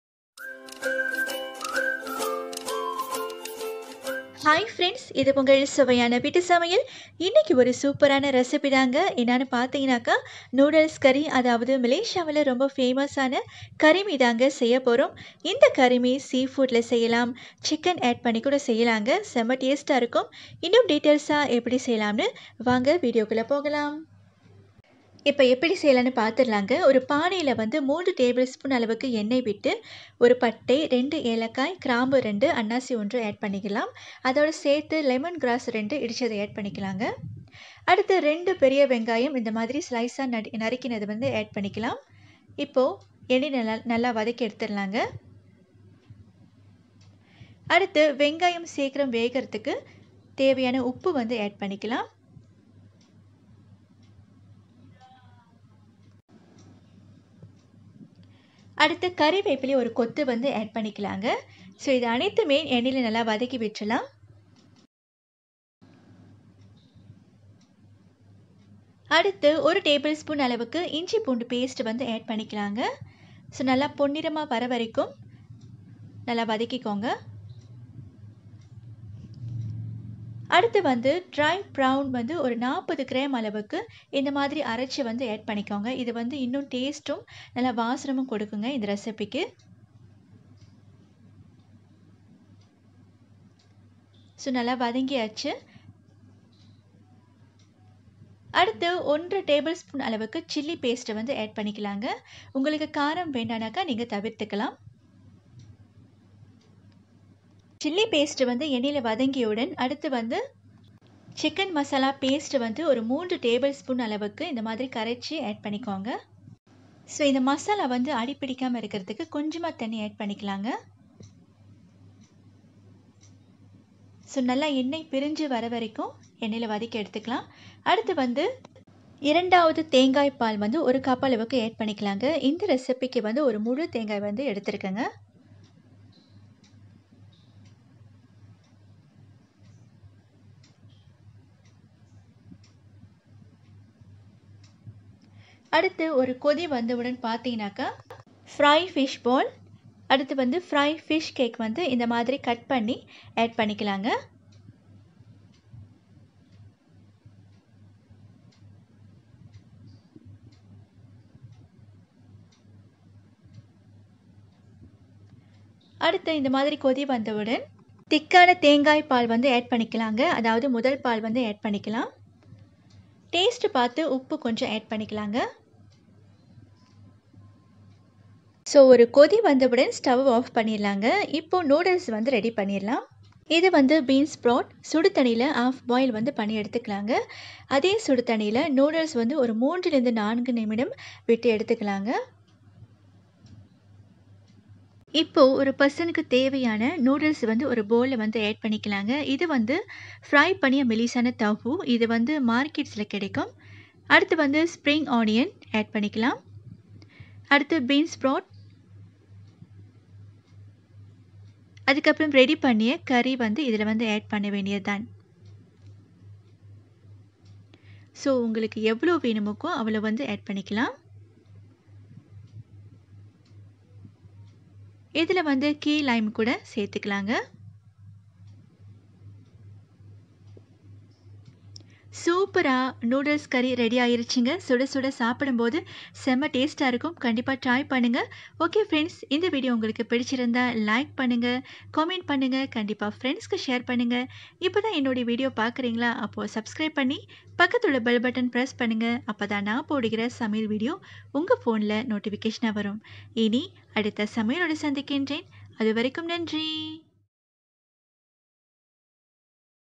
Curry Noodles | Mee Curry | Malaysia Famous Food | கறி னூடல்ஸ்
welcome to Suvaiyana Veetu Samayal channel...
Mee Curry Recipe ~

(this recipe for 5 people)

noodle : 1 packed
chinnamon :1
star anise: 1
cardamom :2
clove: 2
ginger garlic paste :1 tbs
onion 2
lemon grass
curry leaves
chilli paste : 2 tbs
mutton curry powder/ chicken curry powder:3 tbs
dry prawns paste (40 gm)
coconut milk (1 coconut)
fish ball
fish cake
bean sprouts
oil